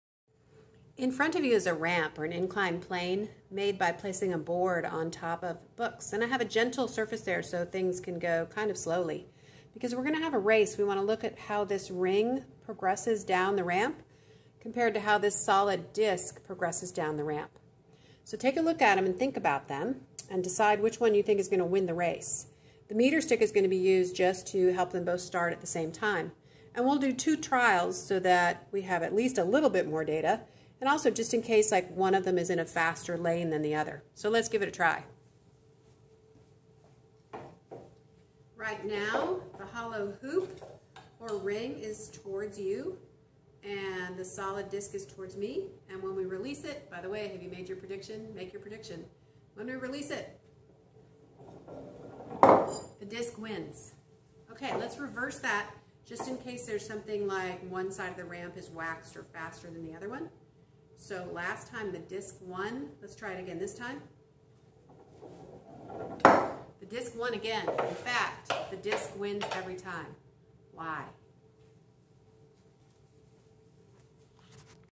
1st Race Hollow ring races sold disk down a ramp, from rest
Rotational Inertia Experiment in MT403